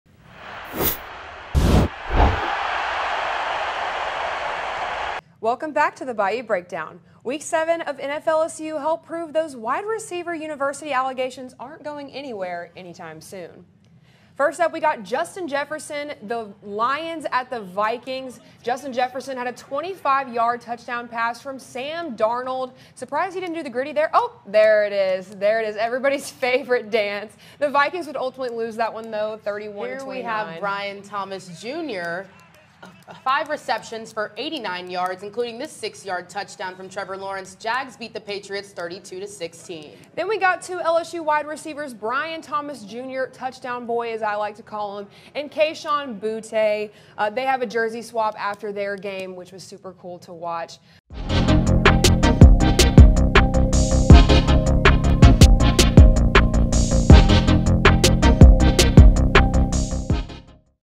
NFLSU week No.7: Breaking down big plays from LSU alumni
Week No.7 of NFLSU has came and left but proved that LSU wont be beating those wide receiver university allegations anytime soon.